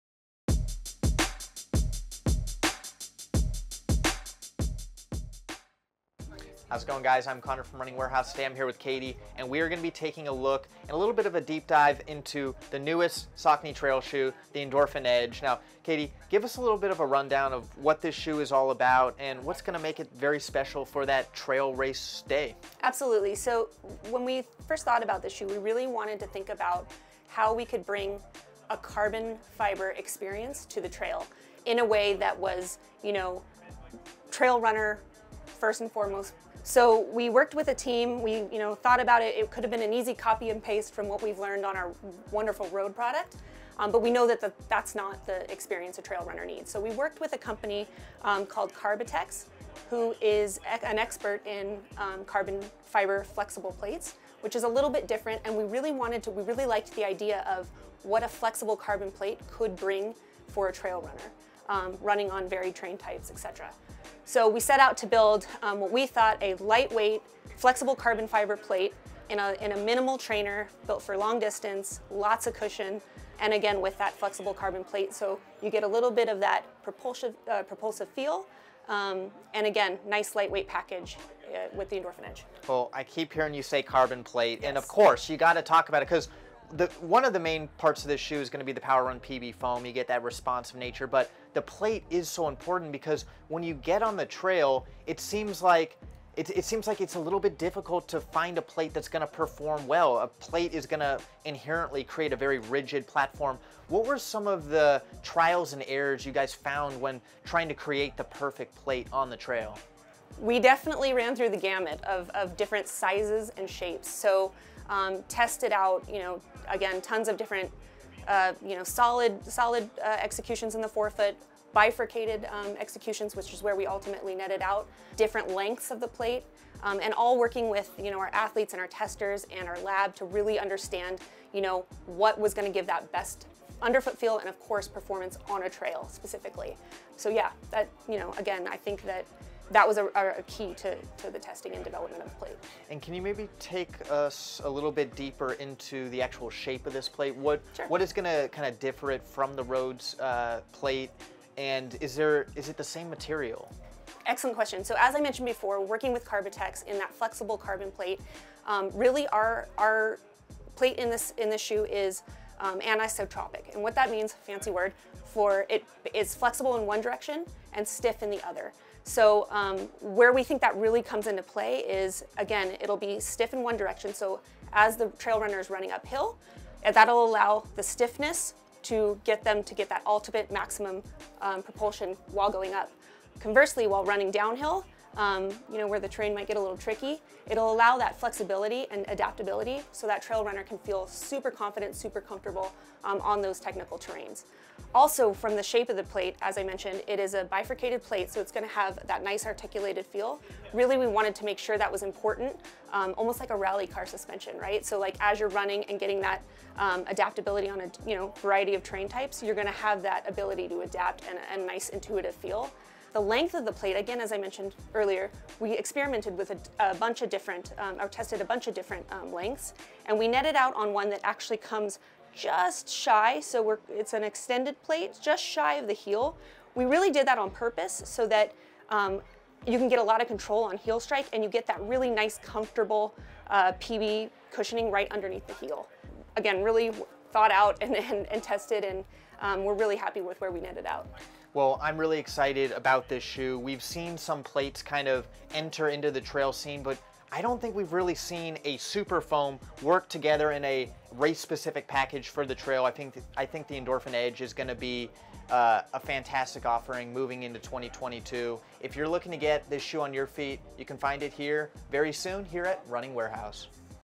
The Carbon Fiber Plate Found Inside the Saucony Endorphin Edge | Top Trail Racing Shoe 2022??
The Saucony Endorphin Edge brings carbon plated super shoe technology to the trails. But how do you go about designing a carbon fiber plate on uneven trail surfaces? We met up with the Saucony Team to see what went into the unique plate design!

Saucony Endorphin Pro 3 vs Saucony Endorphin Speed 3 vs Saucony Endorphin Shift 3 | Best Saucony Running Shoes??